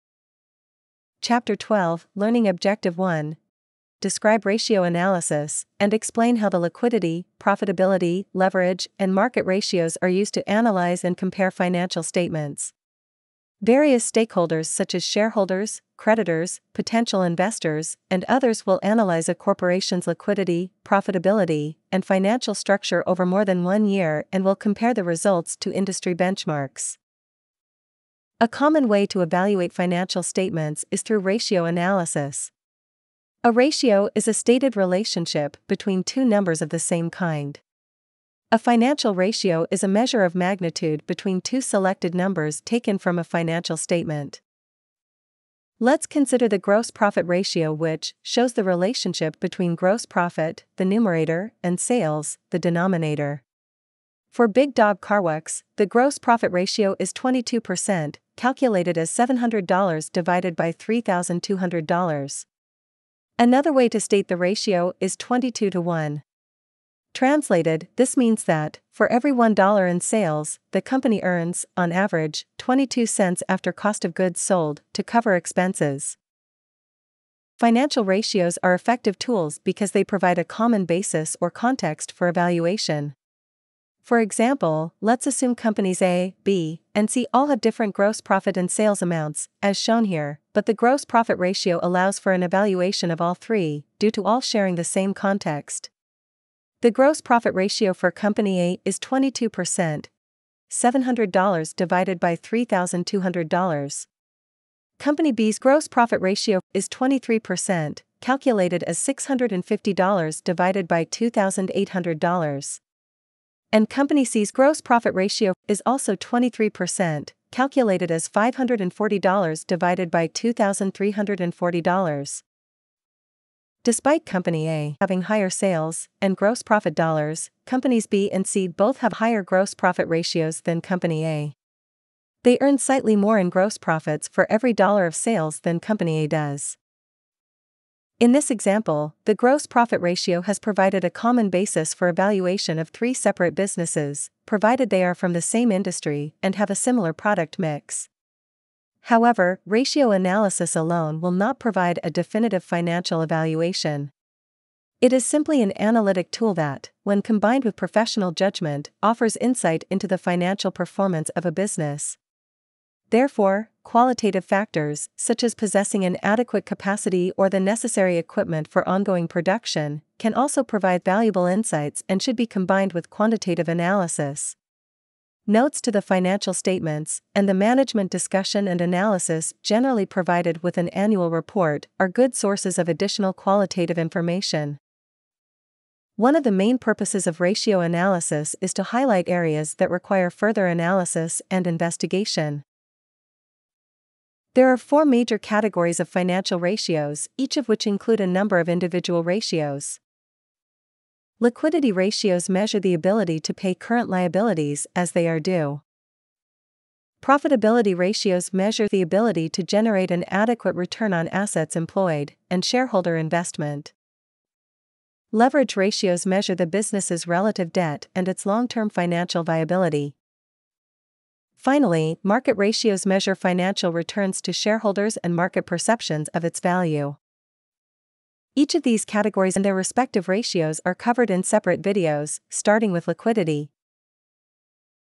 Learning Objective 12.1: Introduction to Ratio Analysis
Chapter 12: Financial Statement Analysis
Learning Objective 1: Describe ratio analysis, and explain how the liquidity, profitability, leverage, and market ratios are used to analyze and compare financial statements.